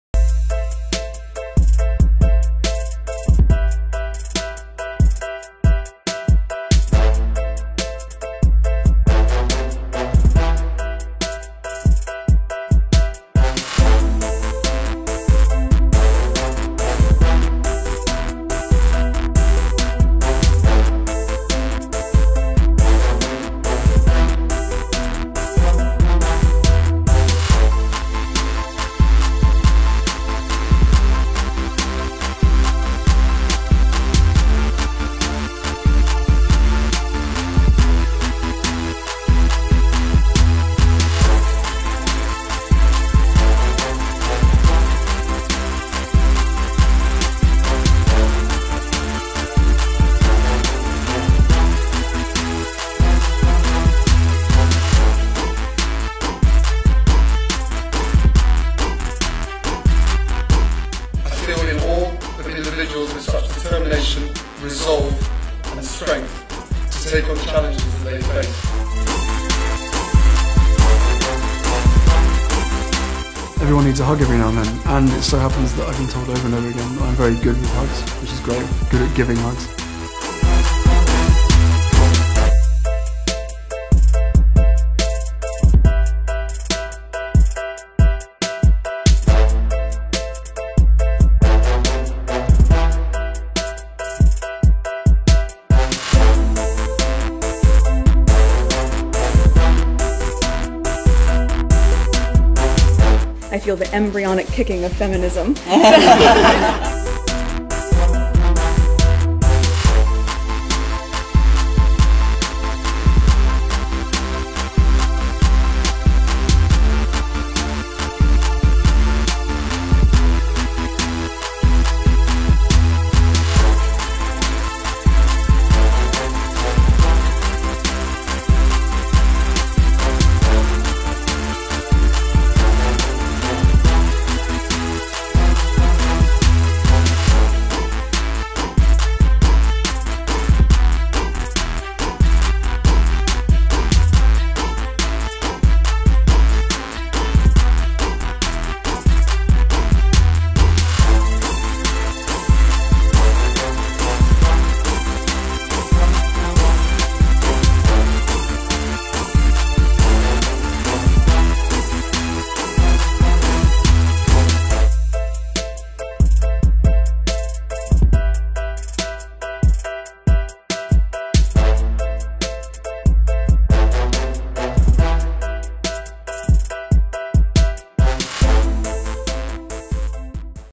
Proof That Harry & Meghanll Be Great Parent
Baby Sussex is almost here! And Harry and Meghan are going to be such wonderful parents. Here's
why.